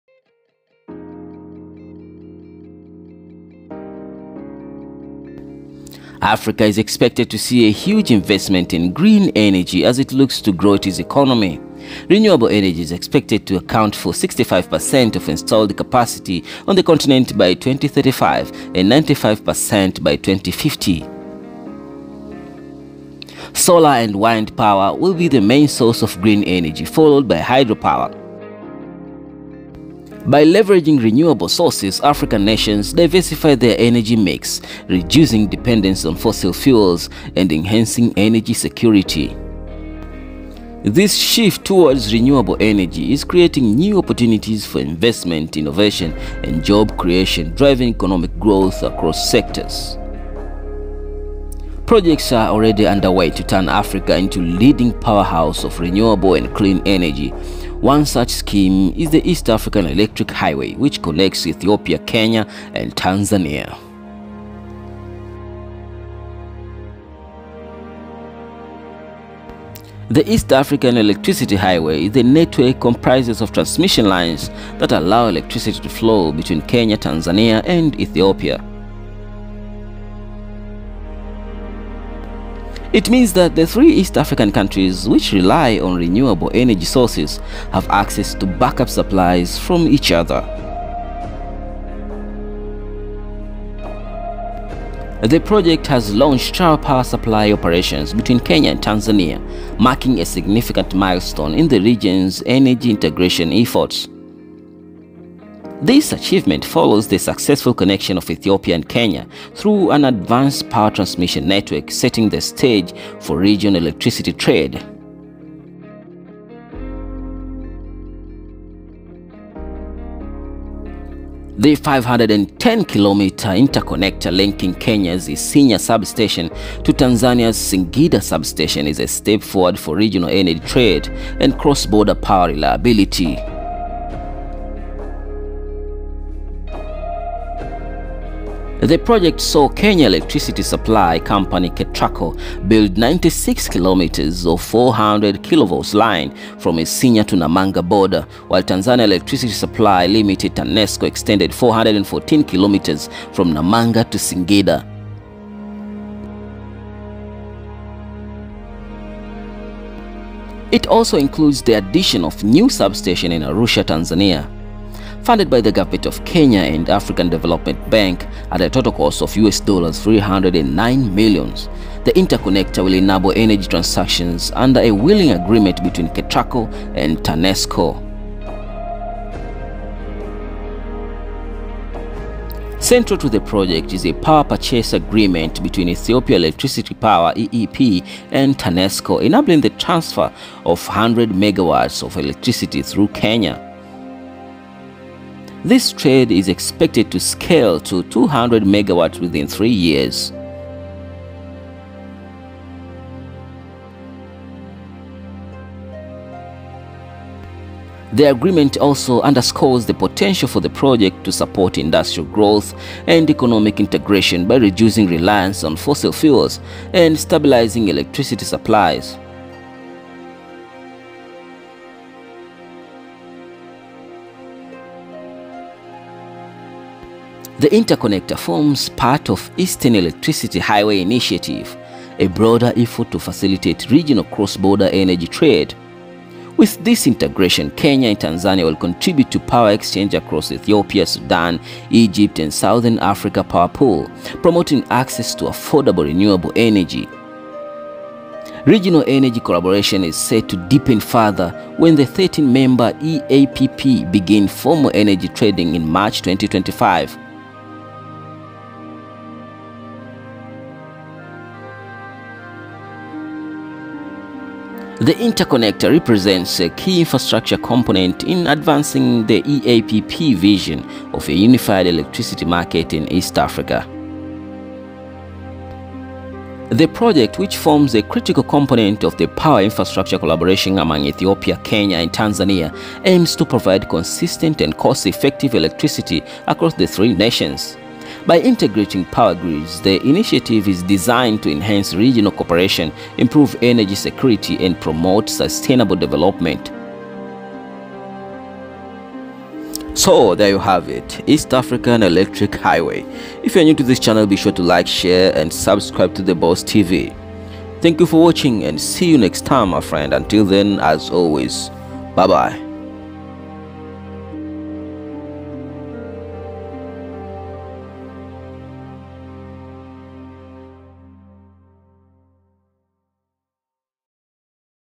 East Africa's Electric Highway REVOLUTION Connecting Kenya Tanzania and Ethiopia!
The electricity highway between Ethiopia and Kenya represents a technological advance for electricity transmission.
The interconnector forms part of the Eastern Electricity Highway initiative, a broader effort to facilitate regional cross-border energy trade. With this integration, Kenya and Tanzania will contribute to power exchanges across Ethiopia, Sudan, Egypt, and the Southern Africa Power Pool, promoting access to affordable renewable energy.

Like this video if you want to see more nice contents and react in the comments section below.